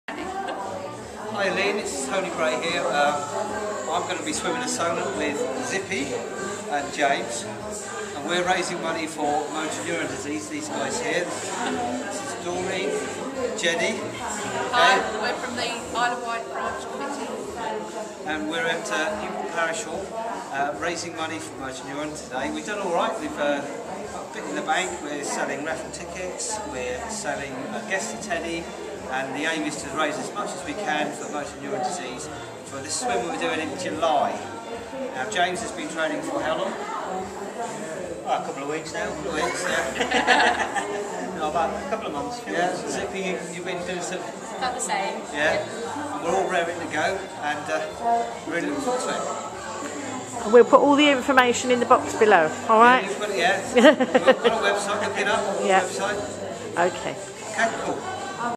The Sponsored Solent Swim fundraiser
Tony Bray and Zippy and James are going to swim the Solent - to raise money for Motor Neurone Disease.